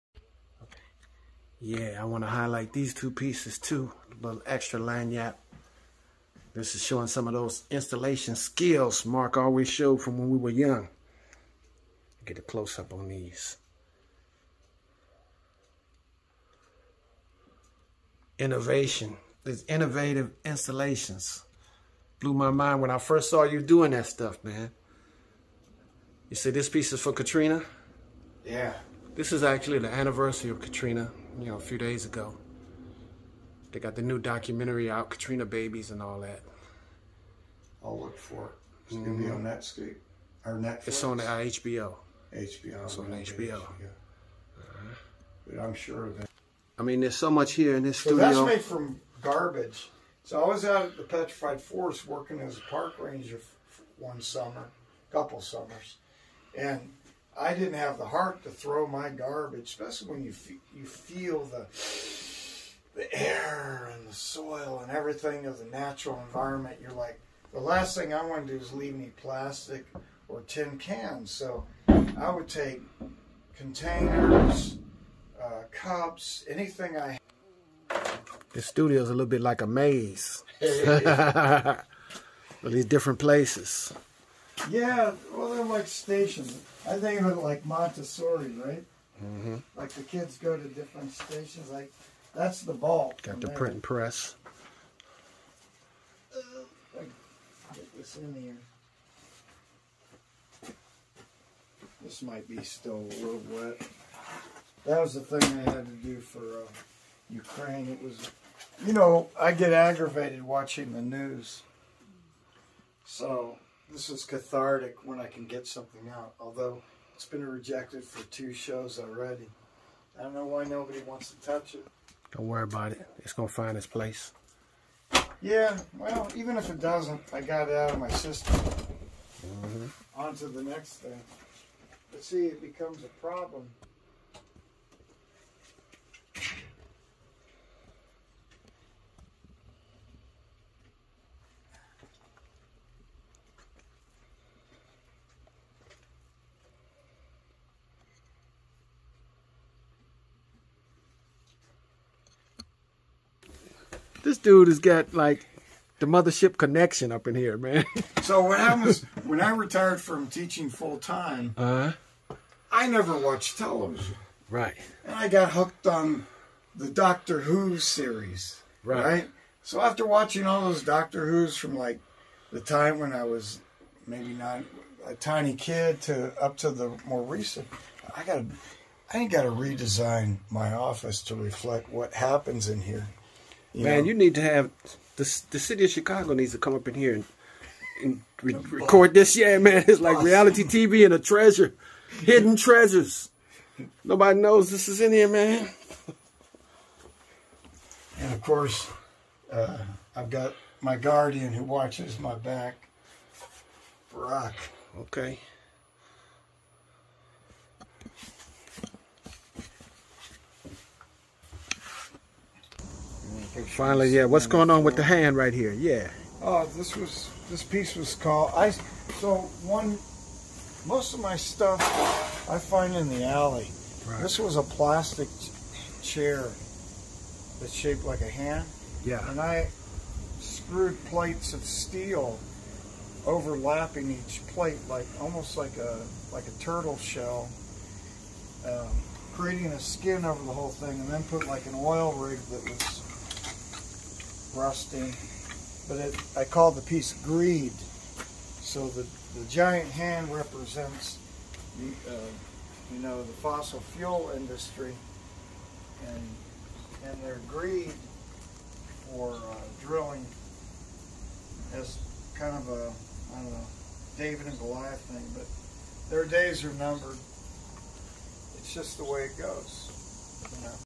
Part 2) A Visit with MARK NELSON at GRINGOLANDIA STUDIO
Bless UP ⬆️⬆️⬆️
I got called on very short notice for 🦺 emergency Olòrìṣà work...
I was in the Shrines they are working behind the scenes for the community and really didn't get a chance to see very many people... But I did make it to Mark Nelson's Studio... He's DOING BIG THANGS up there

Cheque LO ✅

AKINLANA ART
Feel the bRushhhh!!!🖌️🎨🖼️ @marcusakinlana

Cheque LO!